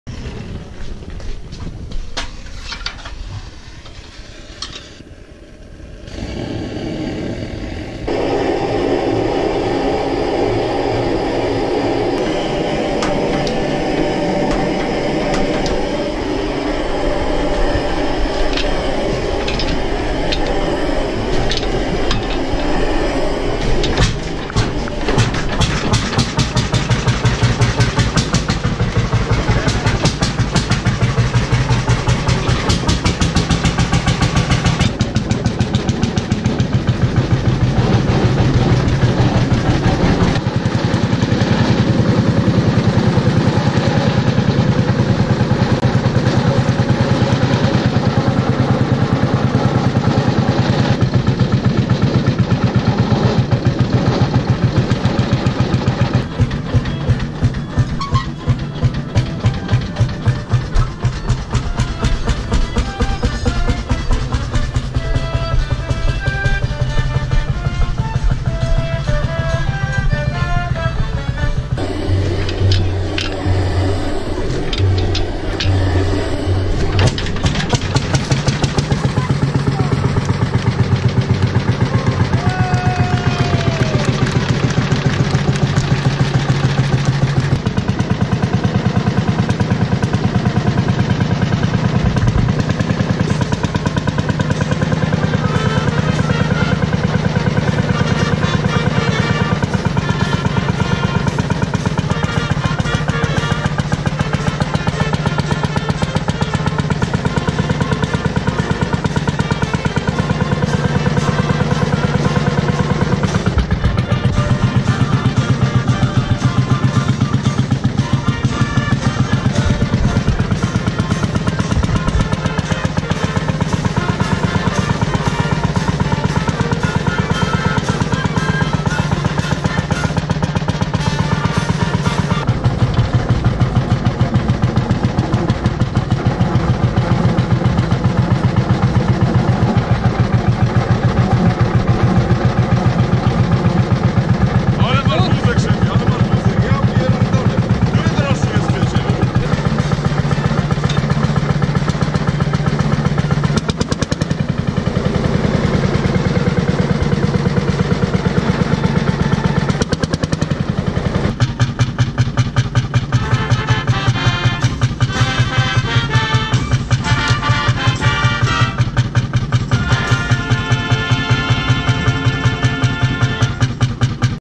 Folkobranie oczami traktorzysty Ursus C45 Lanz 4K
Zostałem zaproszony na Folkobranie 2017 do Kozienic.
Film przedstawia krótką relację z tej uroczystości.
Oczywiście wszystko nagrywane z traktora Ursus C45.
Piękna impreza super klimat ale zerknijcie sami.